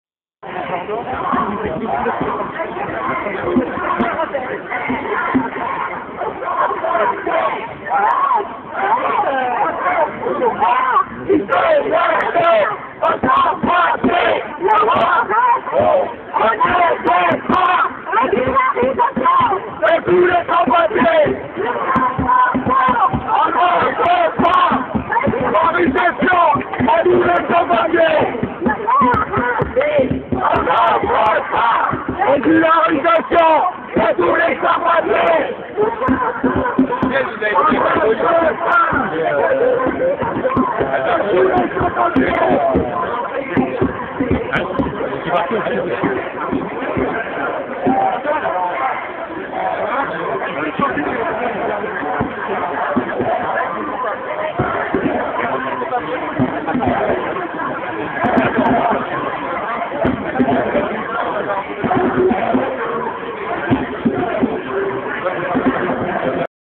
contre la loi loppsi 2 devant le sénat 15 janvier 2011
Contre la loppsi 2 (loi d'orientation et de programmation pour la performance de la sécurité intérieure) loi  liberticide qui solde nos libertés. Danger public, non à la vidéo surveillance, contre la loi sécuritaire et liberticide, aris le 15 janvier du métro Odéon au Châtelet, devant le Sénat.